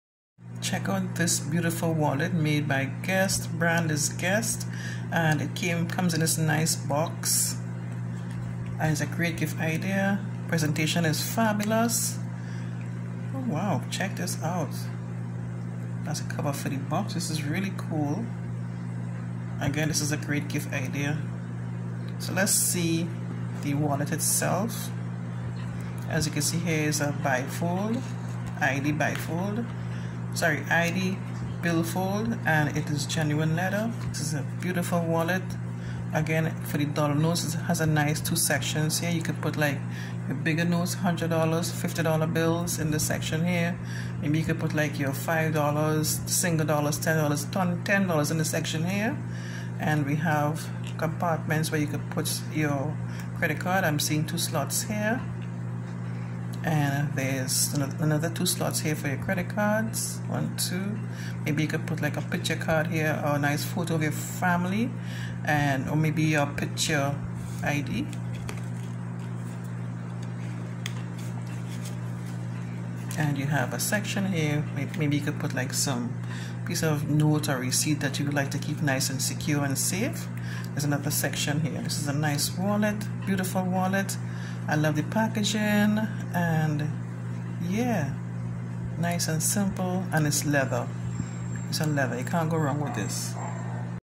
Guess Men's Leather Bifold Wallet Great Gift Idea For Any Occasion.
The simple and straightforward slim bifold features a no fuss design easily fits in your pocket or bag to keep your essentials accessible wherever you go.

This wallet contains 6 card slots, a clear ID window, 2 slip pockets and a currency pocket to keep your essentials secure and organized.

GUESS Men's Leather Slim Bifold Wallet

My storefront with other Amazon Reviews